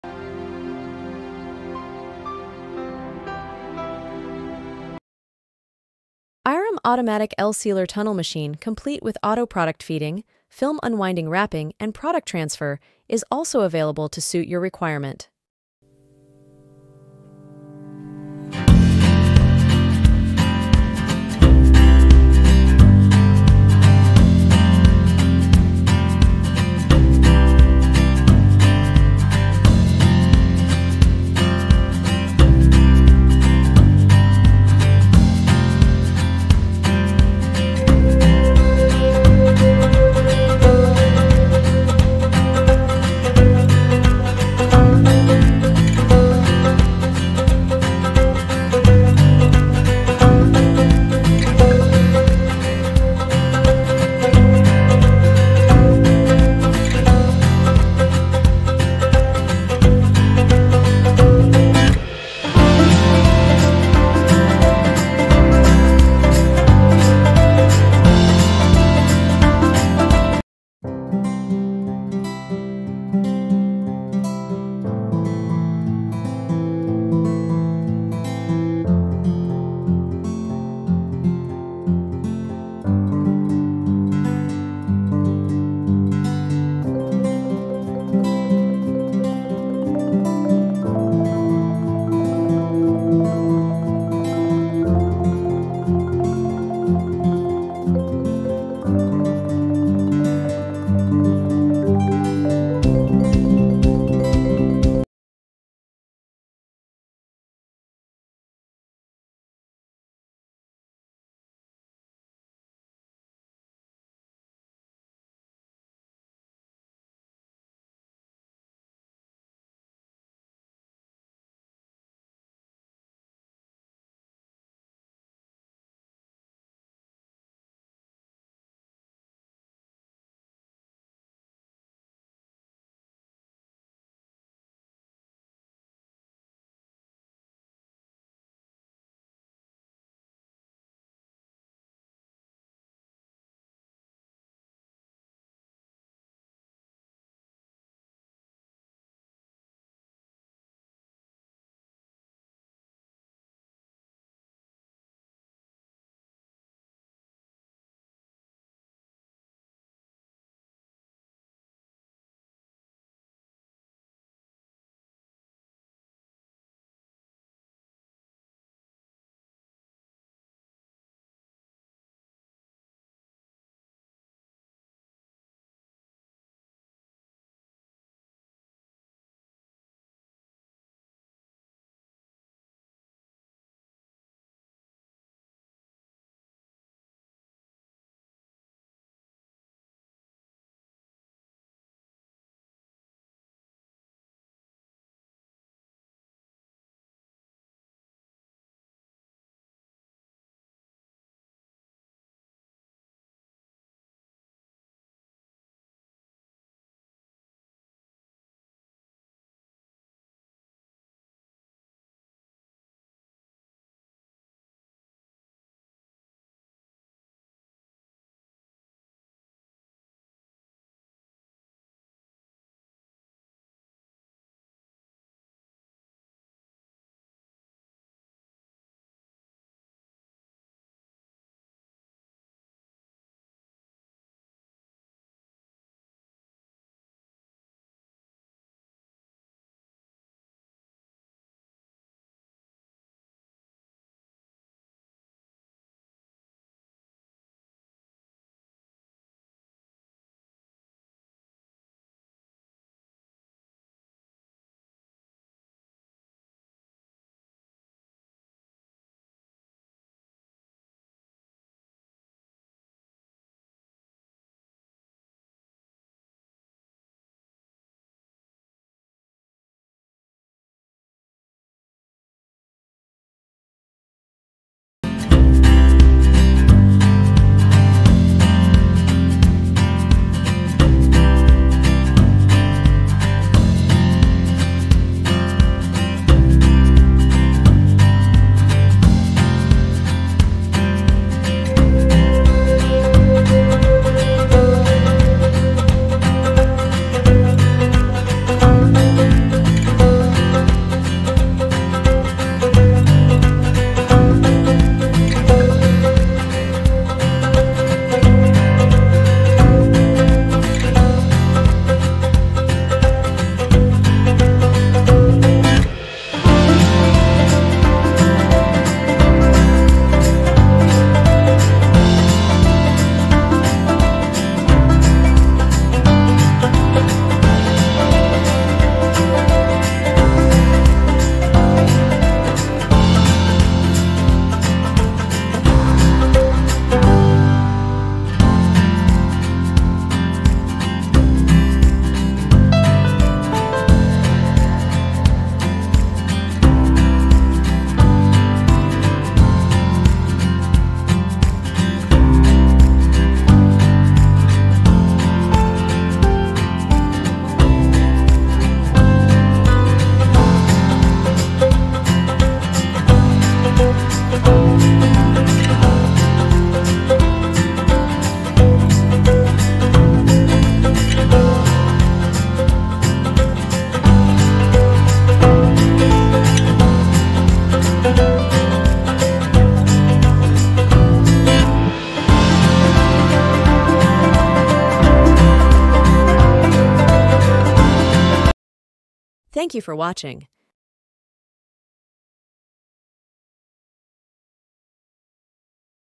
Auto L Sealer Tunnel Machine.
Say goodbye to manual packaging hassles with our Automatic L-Sealer Tunnel Machine! Designed for efficiency and precision, this advanced system effortlessly seals and shrinks your products, saving you time and labour costs. Whether you're packaging pharma, beverages, electronics, mono cartons, or any other product, our machine ensures consistent and professional results every time. Watch our demo to see how easy it is to revolutionize your packaging process with automation!